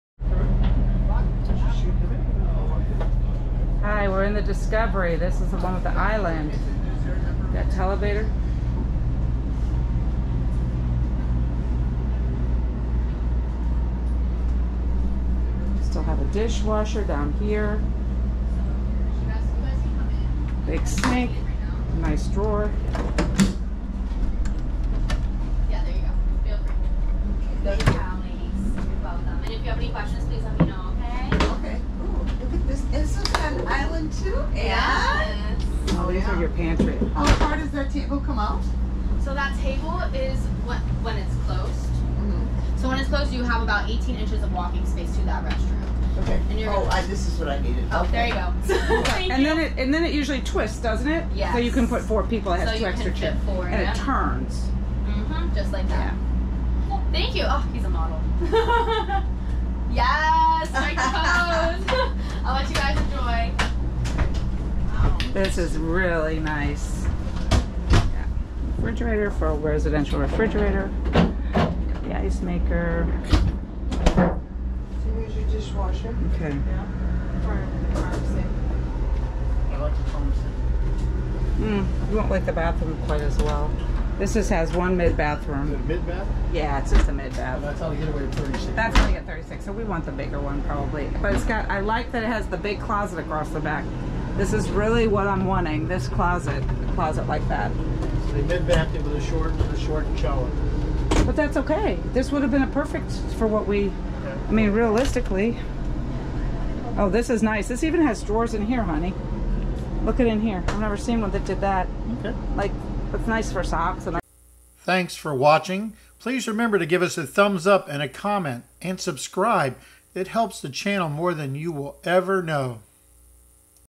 The 2023 Fleetwood Discovery LXE 36HQ.
The 2023 Fleetwood Discovery LXE 36HQ. The Class "A" with the kitchen island and mid bath. That is  only 36 foot long! Who says you can't get into the small places with a class "A".